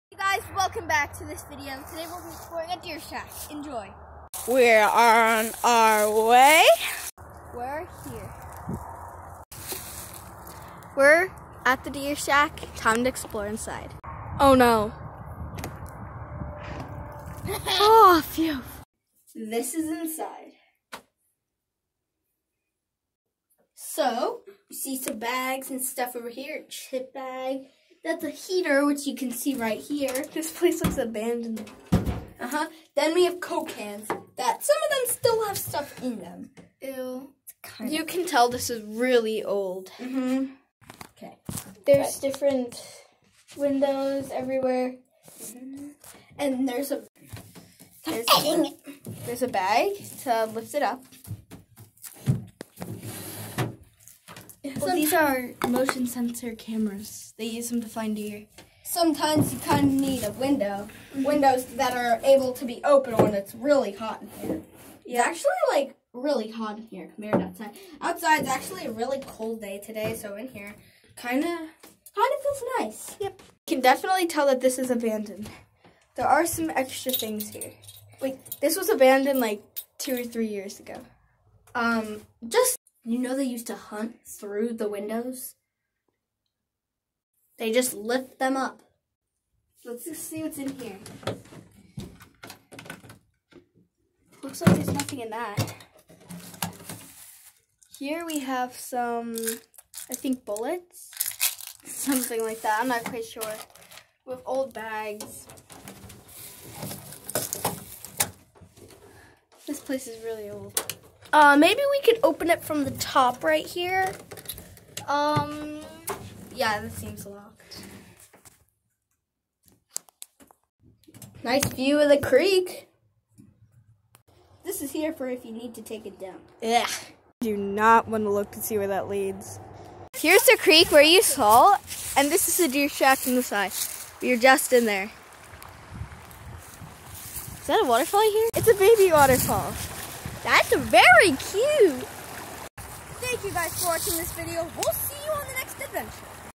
EXPLORING ABANDON HUNTING STAND!!
Hope you enjoy:) We did everything safe and made sure there was nothing dangerous.